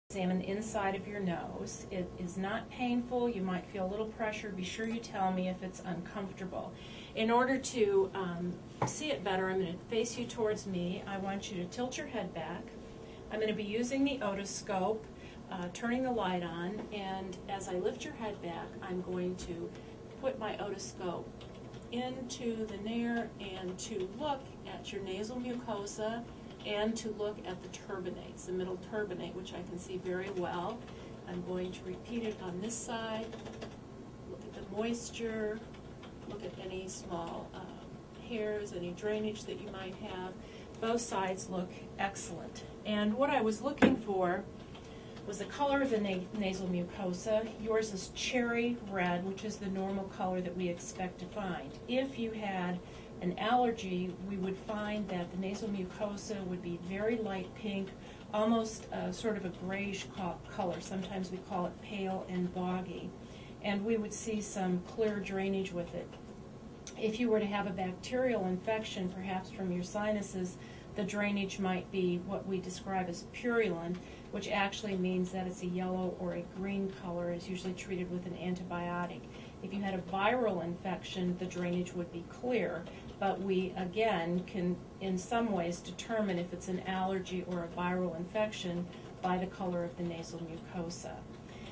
Nose exploration with otoscope
How to perform an anterior nose exploration using otoscope